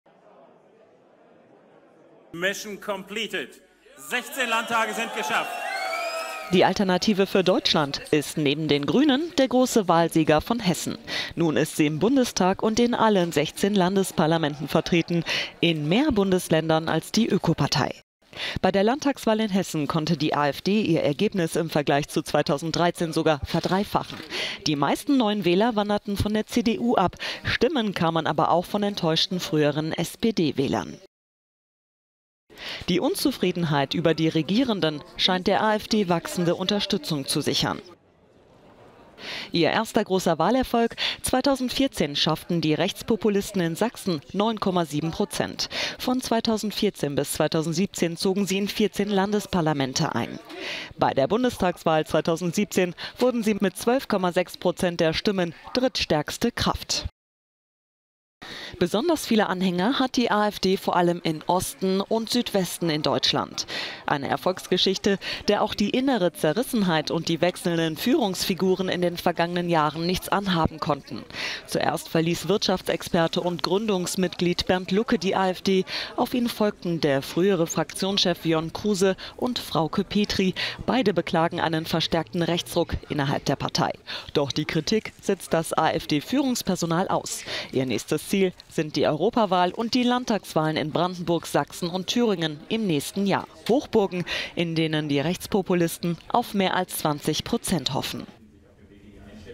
Der Aufstieg der AfD
Die Alternative für Deutschland ist neben den Grünen der große Wahlsieger von Hessen. Nun ist sie im Bundestag und in allen 16 Landesparlamenten vertreten, in mehr Bundesländern als die Ökopartei.…